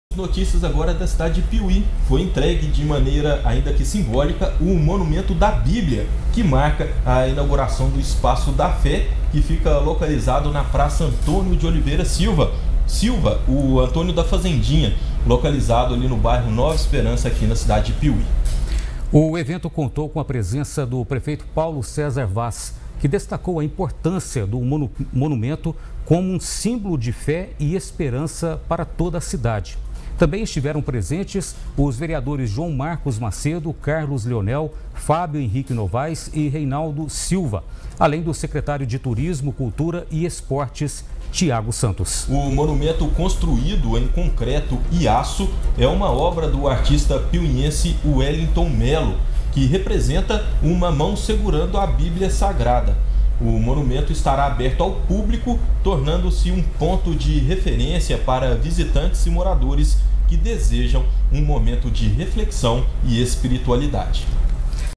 'ESPAÇO DA FÉ' E 'MONUMENTO DA BÍBLIA' SÃO INAUGURADOS EM PRAÇA DE PIUMHI
| PIUMHI |

Foi entregue, de maneira ainda simbólica, o "Monumento da Bíblia", que marca a inauguração do Espaço da Fé, localizado na praça Antônio de Oliveira Silva (Antônio da Fazendinha), no bairro Nova Esperança, em Piumhi. Veja todos os detalhes: